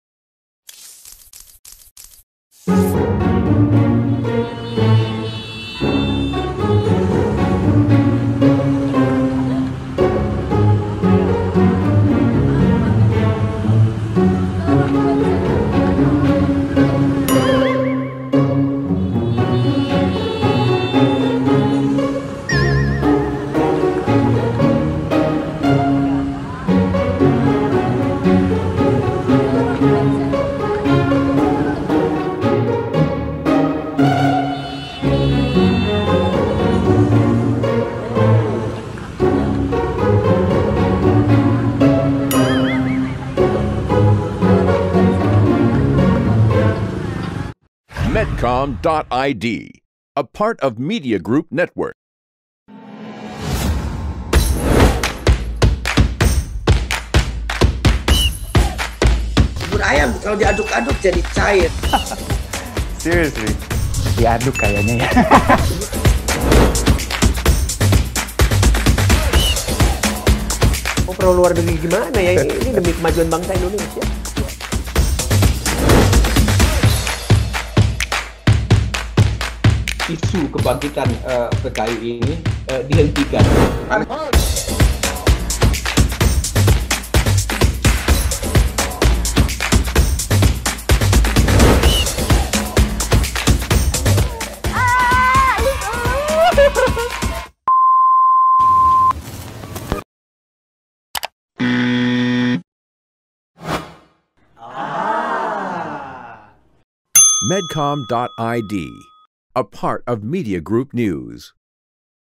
Viral! Wisatawan di Yogyakarta Nekat Foto di Tengah Jalan
Dalam video memperlihatkan seorang wanita tampak berpose di tengah jalan dengan latar Tugu Jogja. Tak berapa lama, puluhan kendaraan yang sebelumnya berhenti di lampu merah terlihat melaju. Wanita tersebut sempat diklakson pengendara yang melintas dan nyaris tertabrak. Lewat unggahannya, pengunggah video mengimbau kepada wisatawan yang berlibur di Jogja untuk berhati-hati dan berharap kejadian tersebut tidak terulang lagi.